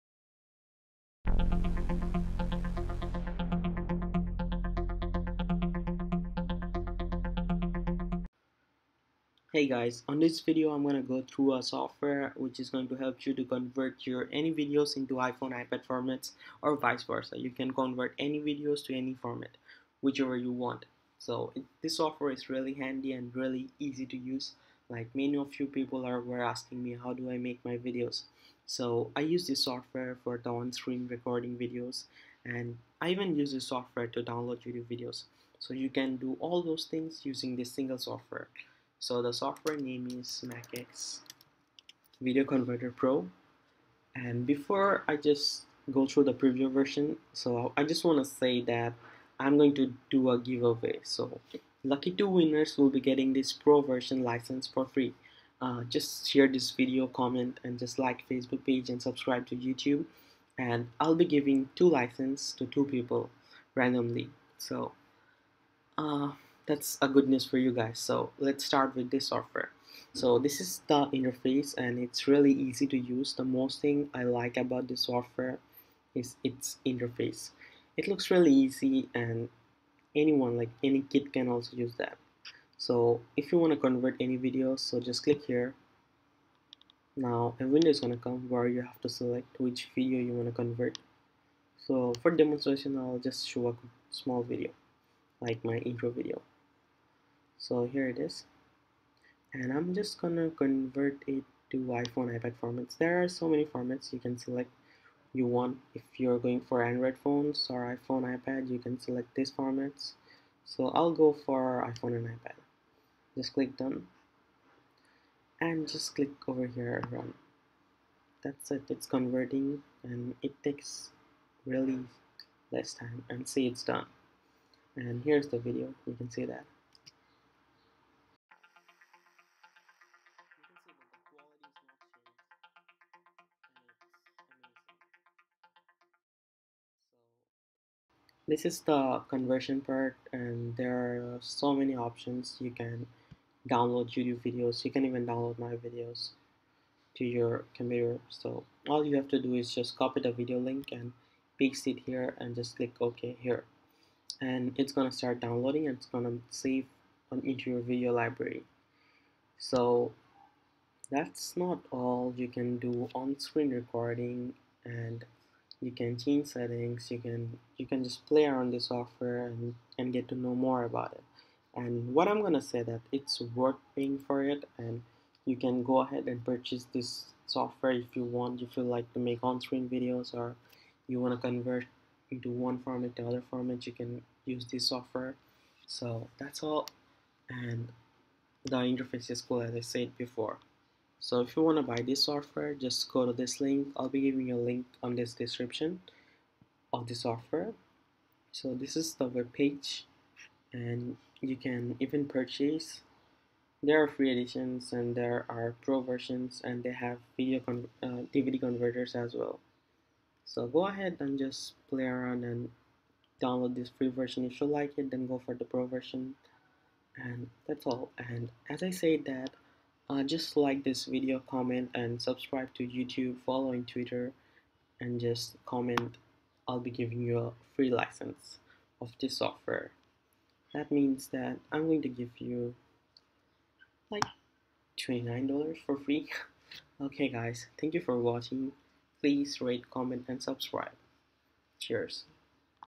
MacX Video Converter Pro-Convert Video to AVI, MP4, MPEG, MOV, iPad, iPhone and Andriod
With wide codec support, MacX Video Converter Pro is an all-in-one video converter software that let helps you to convert any videos to AVI, MP4, MPEG, MOV, FLV, WMV, etc for playback on Mac, iTunes, QuickTime, iPad, iPhone, Android smartphones and tablets, etc.

Welcome to be a part of MacXDVD community.